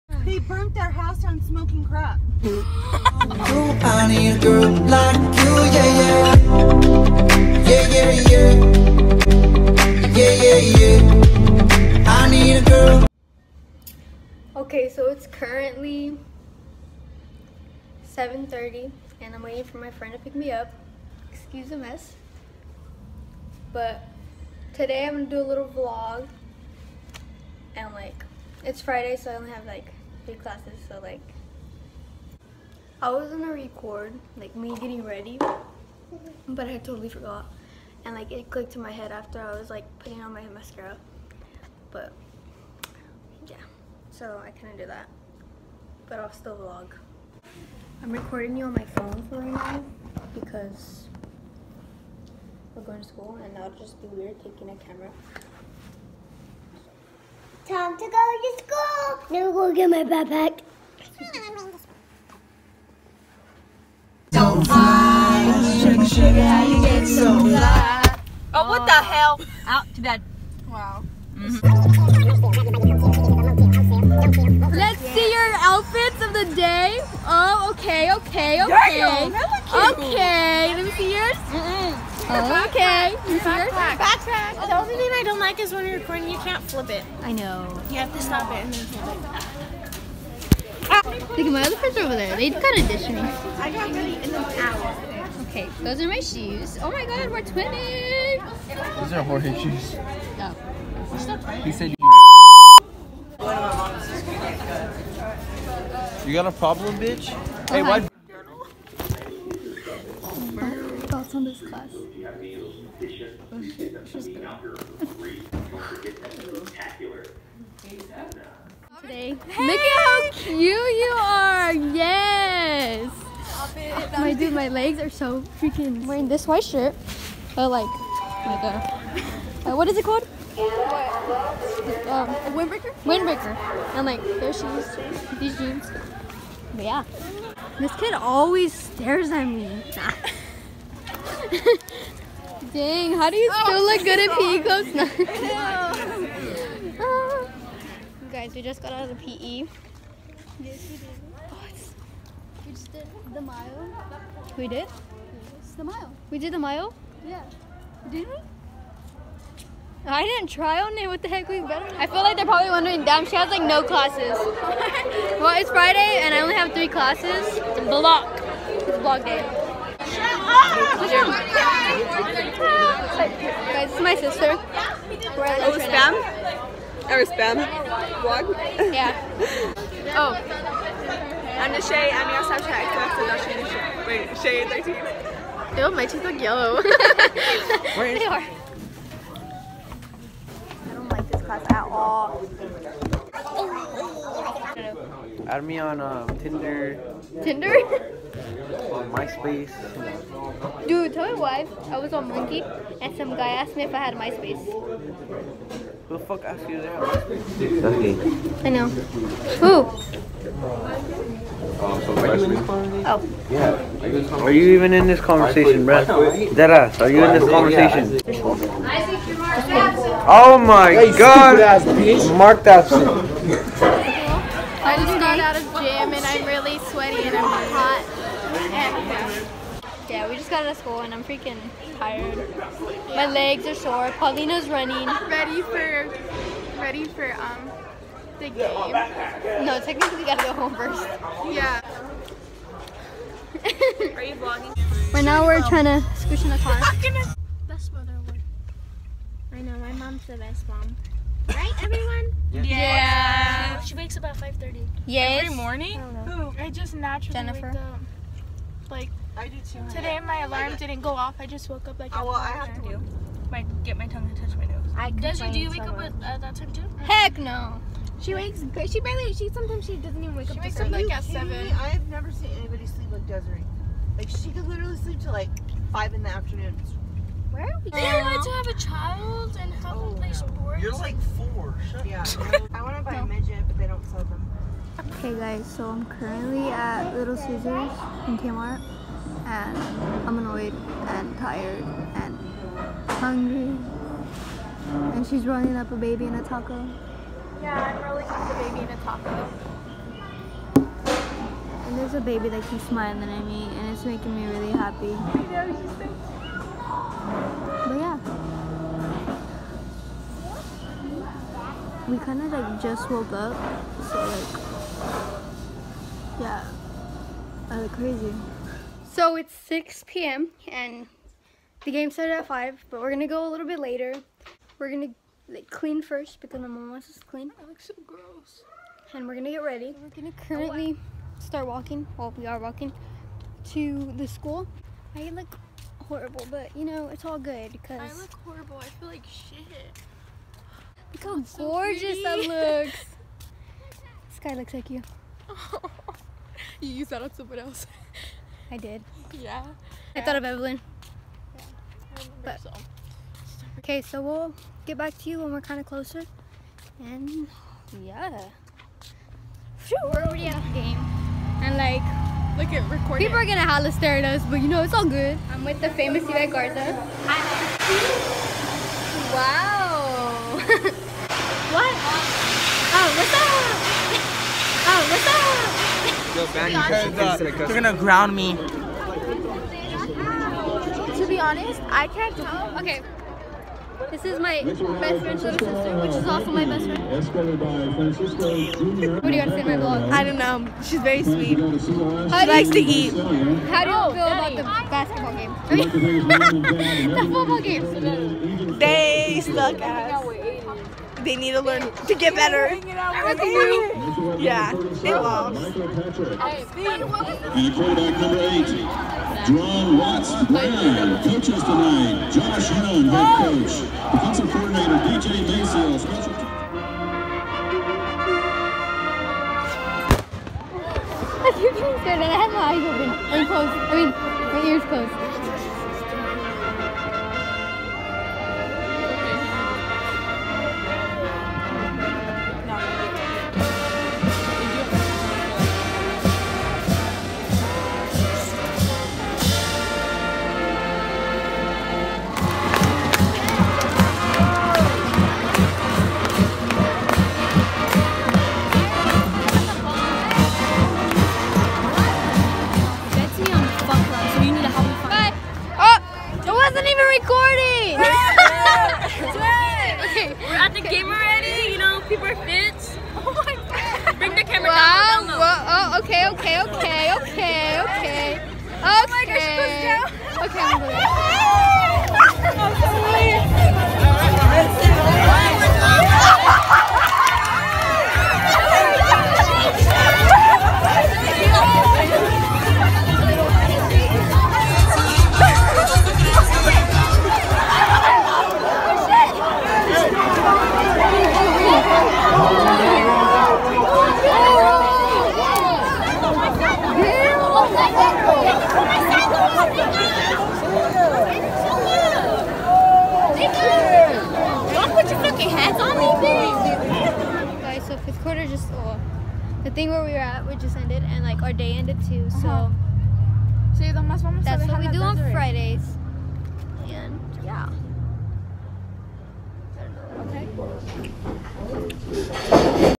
Friday Vlog~ There was a Fight?!?
• Come to school with me, MilkCan Football Game and 5th quarter ( 5 quarter is a dance we have after every home game ! ) 💜💛.
I didn’t do a outtro i was kinda out of it 😂